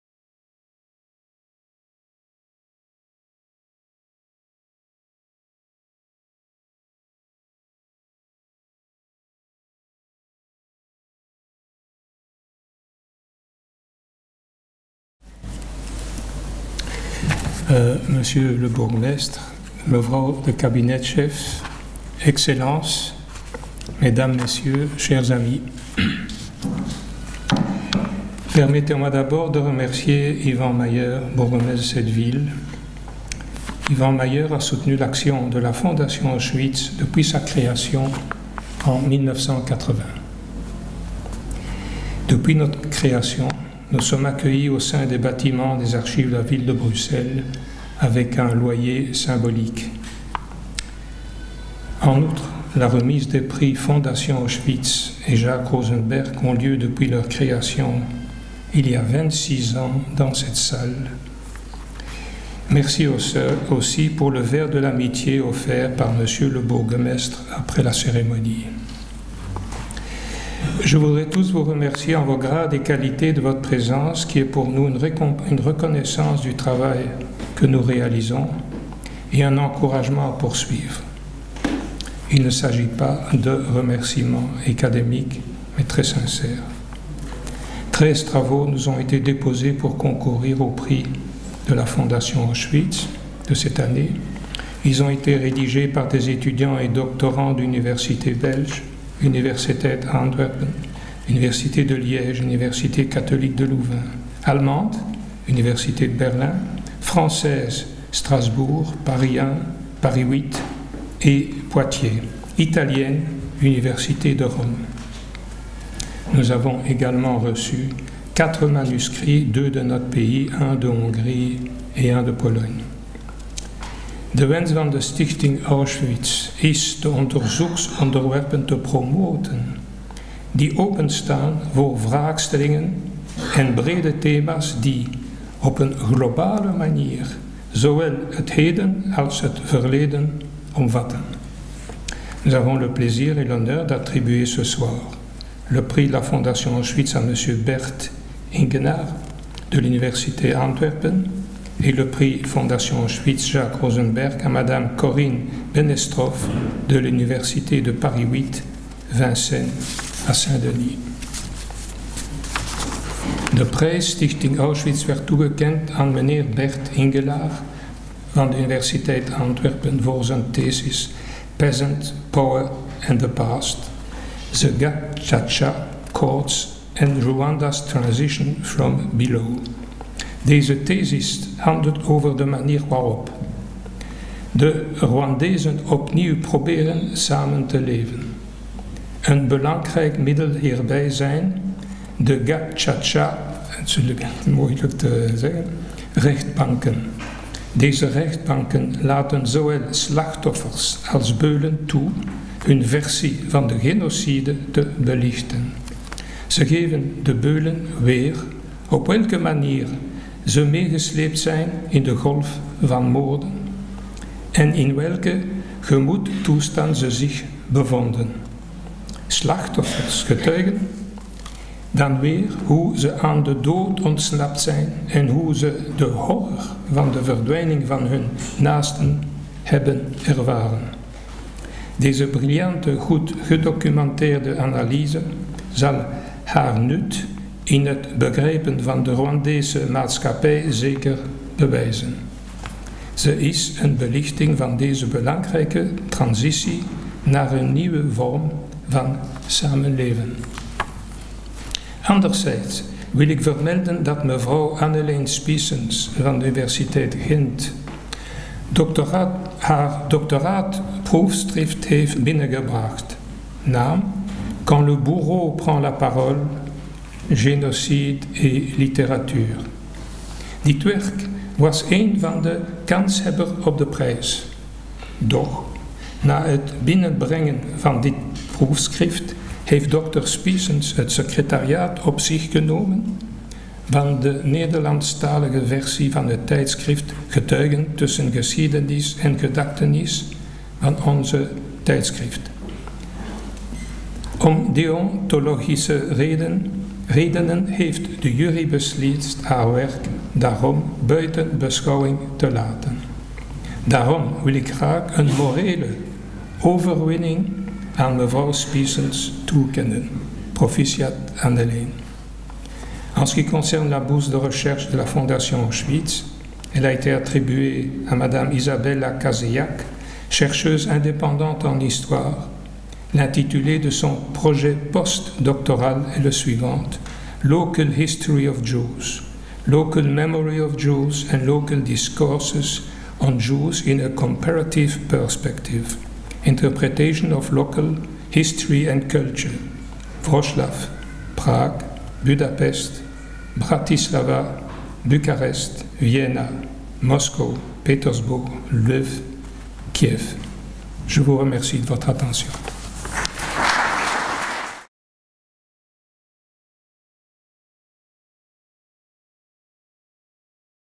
H. Goldberg – Président de la Fondation Auschwitz - 2014-10
Monsieur Henri Goldberg, Président de la Fondation Auschwitz et de la Mémoire d’Auschwitz ASBL.

La Séance académique de remise des Prix de la Fondation Auschwitz a eu lieu dans la Salle gothique de l’Hôtel de Ville de Bruxelles le 9 octobre 2014.

Les Prix de la Fondation Auschwitz – qui récompensent annuellement, en hommage à toutes les victimes des camps de concentration et d’extermination nazis, deux travaux universitaires de haut niveau – ont été attribués pour l’année académique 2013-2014 aux chercheurs suivants :

Le « Prix Fondation Auschwitz » a été attribué à Monsieur Bert INGELAERE pour son travail intitulé Peasants, Power and the Past. The Gacaca Courts and Rwanda’s Transition « From Below », University of Antwerp, décembre 2012.
Le « Prix Fondation Auschwitz – Jacques Rozenberg » a été attribué à Madame Corinne BENESTROFF pour son travail intitulé Résistance et résilience dans l’œuvre de Jorge Semprun, Université Paris 8 Vincennes à Saint-Denis, 2013.

De plus, Madame Patricia NAFTALI, pour son travail intitulé La construction du « Droit à la vérité » en droit international. Une ressource ambivalente à la croisée de plusieurs mobilisations, ULB, 2013, et Madame Valentina VANZINI, pour son travail intitulé La Sociologia di fronte all'abisso della Shoah, universita’ degli studi di Roma « Tor Vergata », 2013, ont reçu le bénéfice de l’article 4 du règlement permettant au Conseil d’Administration de la Fondation Auschwitz de leur allouer, dans le cadre d’une Convention de recherche, un subside pour la rédaction d’un article à publier dans notre Revue Témoigner entre histoire et mémoire, Paris, Kimé ou sur notre site internet.

D'autre part, la « Bourse de recherche de la Fondation Auschwitz » a été attribuée à Madame Izabela KAZEJAK, pour son projet de recherche intitulé Local history of Jews, Local Memory of Jews and Local Discourses on Jewsin a Comparative Perspective – Interpretations of Local History and Culture: Wroclaw, Prague, Budapest, Bratislava, Bucharest, Vienna, Moscow, Petersburg, L’viv, Kiev.

Pour plus d'informations :
Fondation Auschwitz - Mémoire d'Auschwitz ASBL